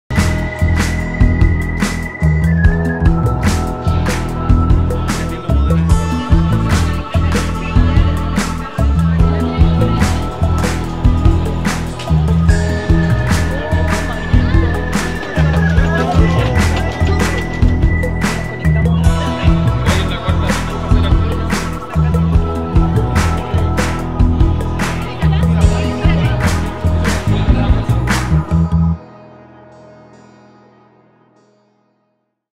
3er Festival de Ingeniería y Ciencias: Jornada familiar
Agradecemos a las más de 15 mil personas que respondieron a nuestra invitación durante el 20, 21 y 22 de octubre. Así termina la tercera versión del Festival de Ingeniería y Ciencias con una jornada dedicada a las familias de la comunidad universitaria, egresados, vecinos y ciudadanía toda.